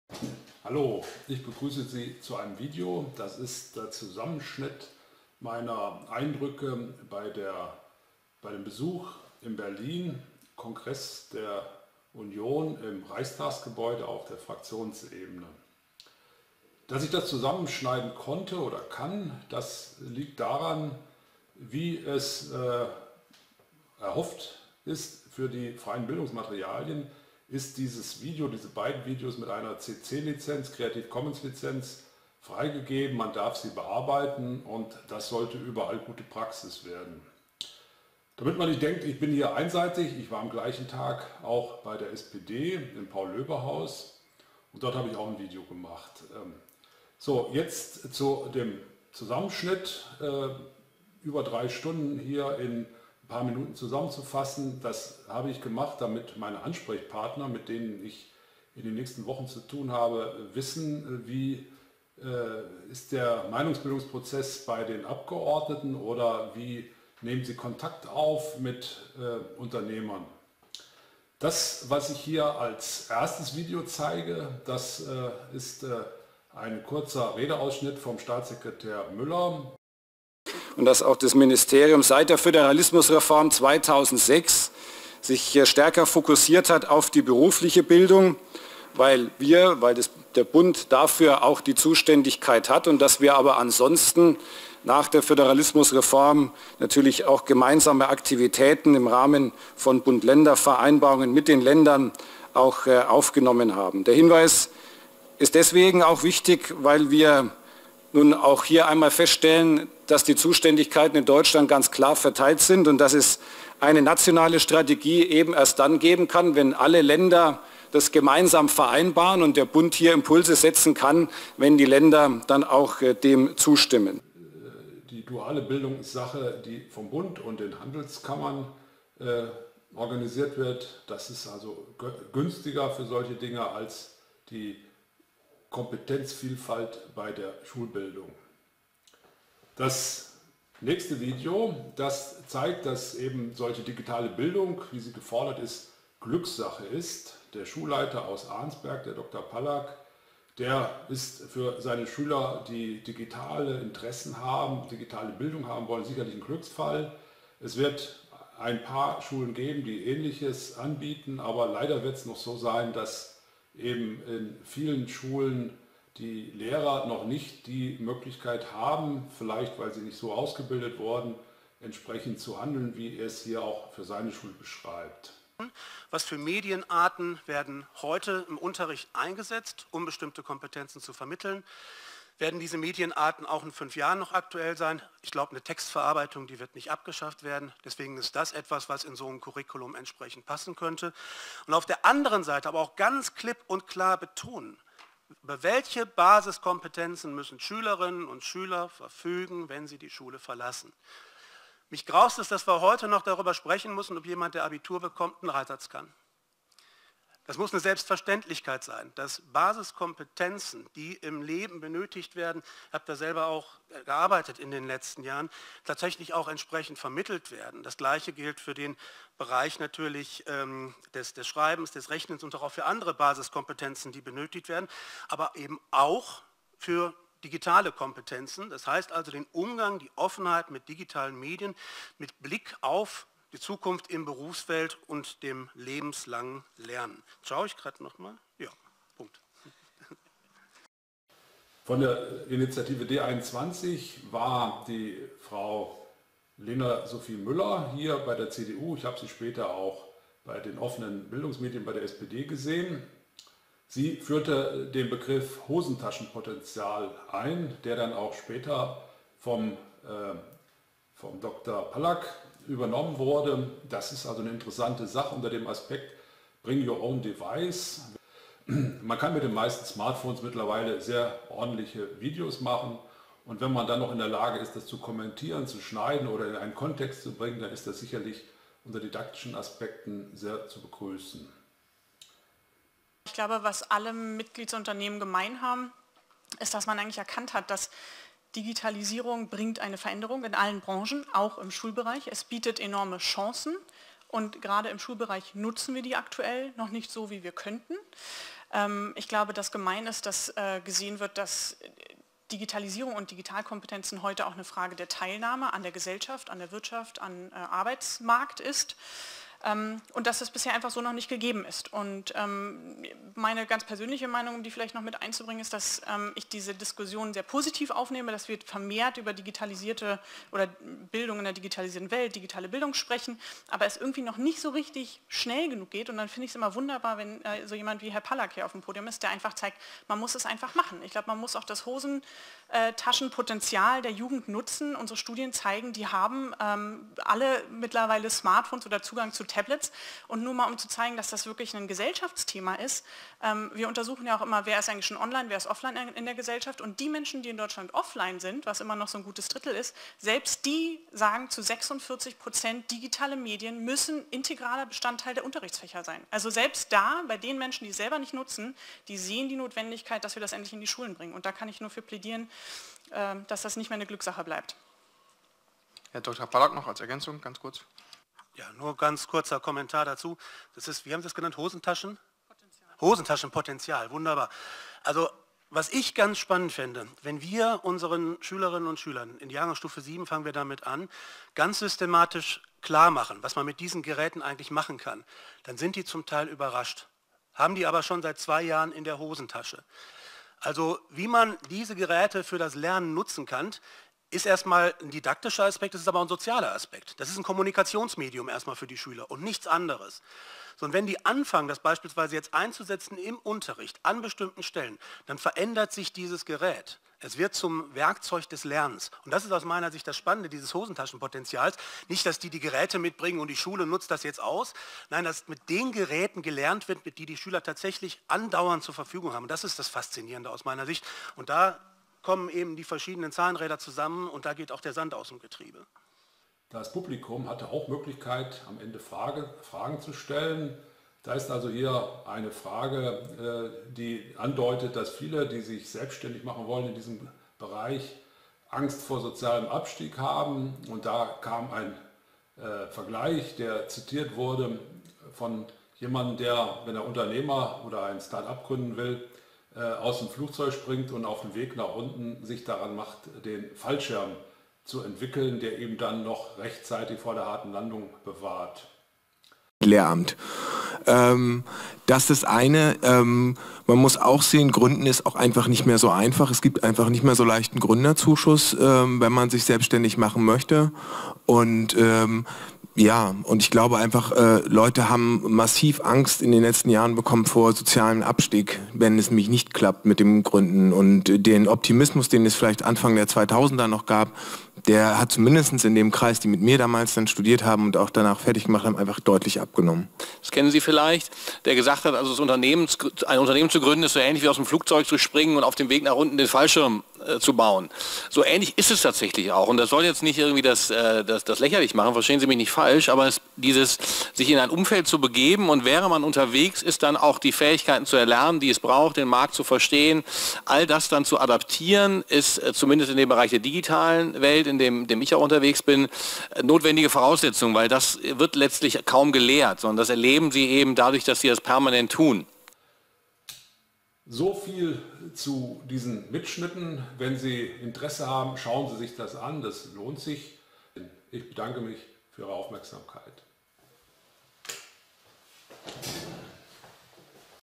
Kongress Digitale Bildung neu denken
Ein Zusammenschnitt zur Vorbereitung von Gesprächen zur Digitalen Bildung und Ausbildung. Details finden Sie in der zugehörigen Pressemitteilung